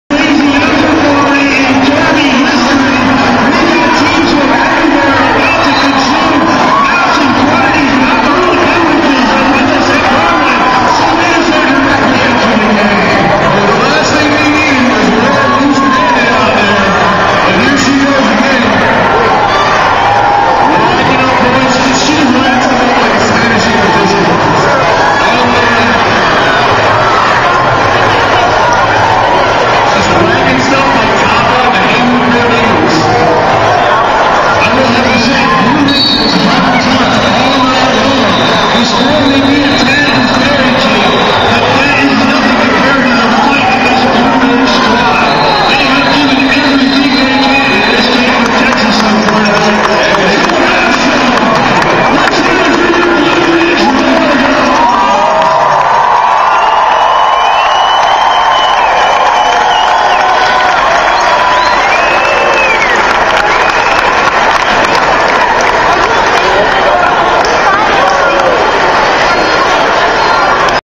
Blue Ridge Rollergirls, July 31 2010 - Game Not Over
On Saturday, July 31st, 2010, the Blue Ridge Rollergirls were pitted against the Texas Hell Marys in a battle of the titans held within the stadium of the Civic Center in downtown Asheville, North Carolina. For more information on the Blue Ridge Rollergirls and their future roller derbies, (Video by Jeffrey DeCristofaro, courtesy of the Blue Ridge Rollergirls)